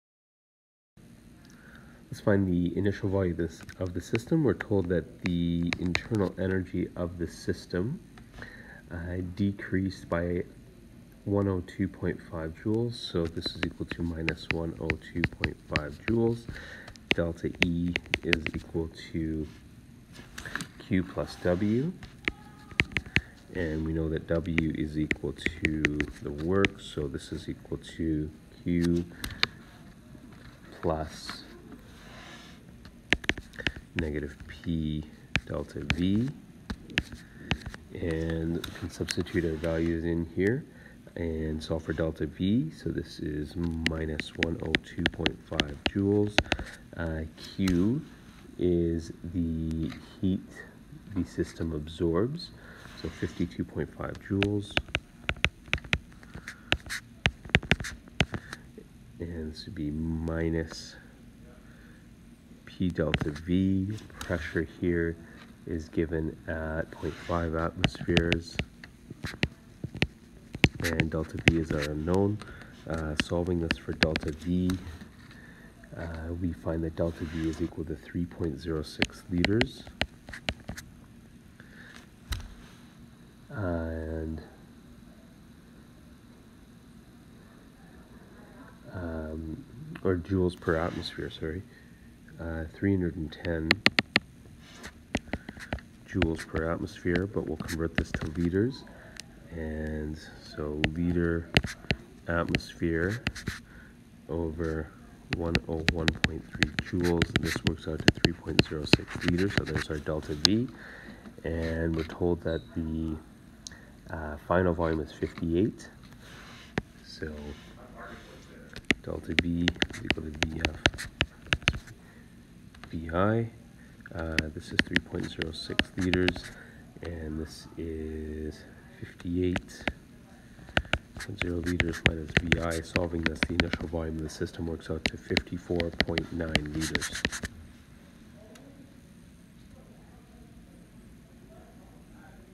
As a system increases in volume, it absorbs 52 5 of energy in the form of heat from the surroundings
As a system increases in volume, it absorbs 52.5 of energy in the form of heat from the surroundings. The piston is working against a pressure of 0.500 The final volume of the system is 58.0 . What was the initial volume of the system if the internal energy of the system decreased by 102.5